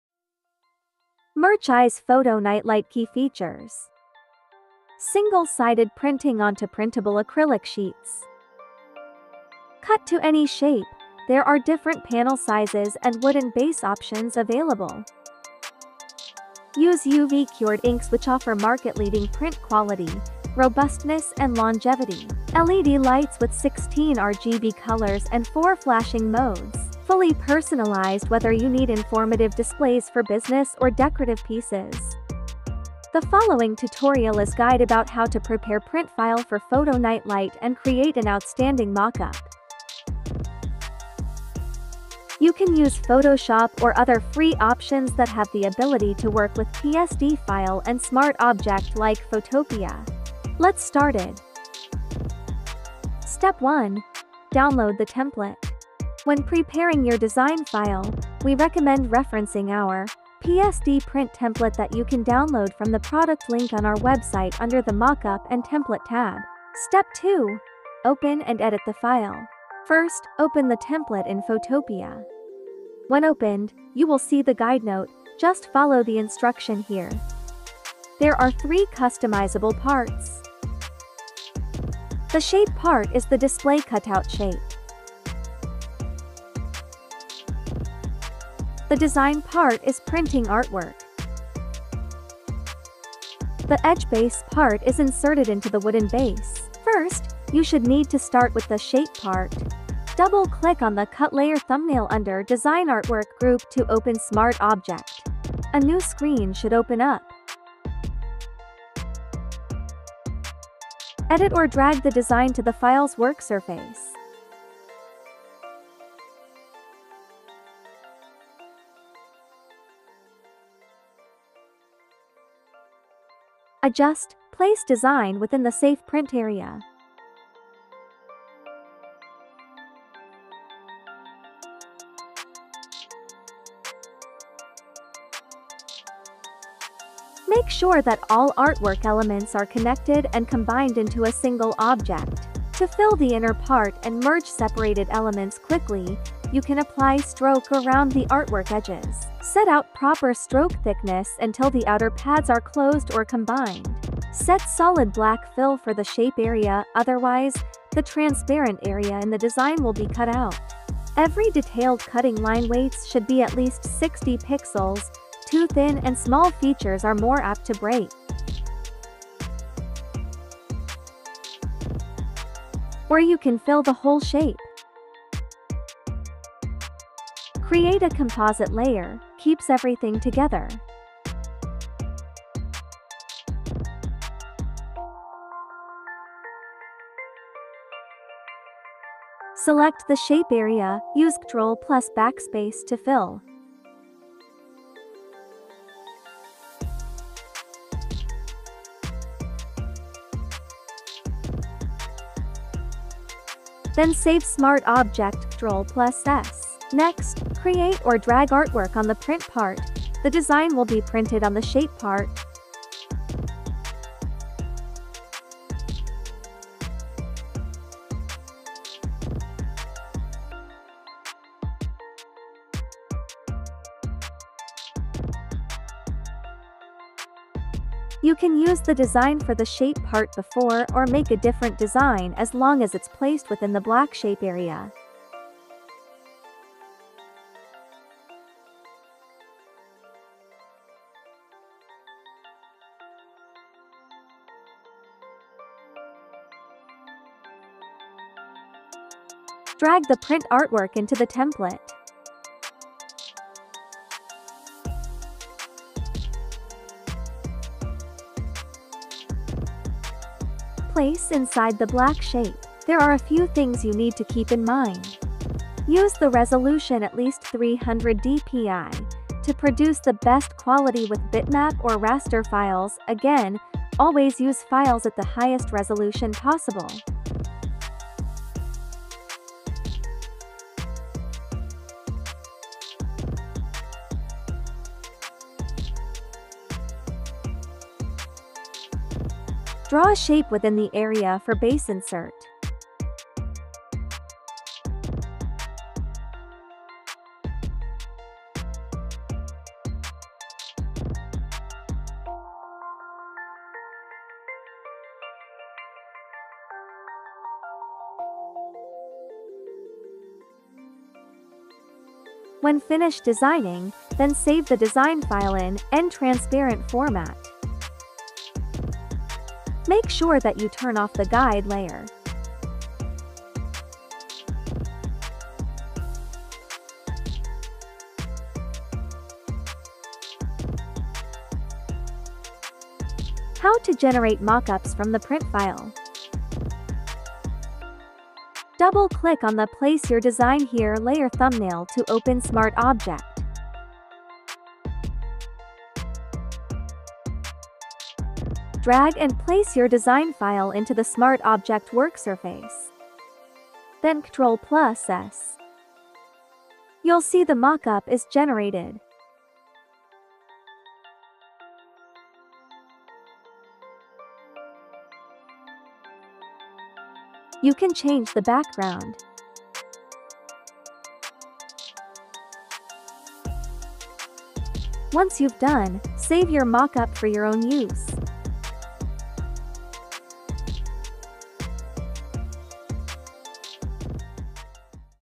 |Merchize Tutorial| Photo Night Light Led Lamp I Template & Mockup Guidelines
The following tutorial is guide about How to prepare print file for Photo Night Light and create an outstanding Mockup.

In this video:
0:00 - Introduction
0:51 - Guide to use the .PSD Templates
05:31 - Guide to generate Mockup

*** Merchize Photo Night Light Key Features:
-- Single Sided: Printing onto printable acrylic sheets.
-- Cut to any shape, there are different panel sizes & wooden base options available.
-- Use UV cured inks which offer market leading print quality, robustness and longevity.
-- LED lights with 16 RGB colors and 4 flashing modes.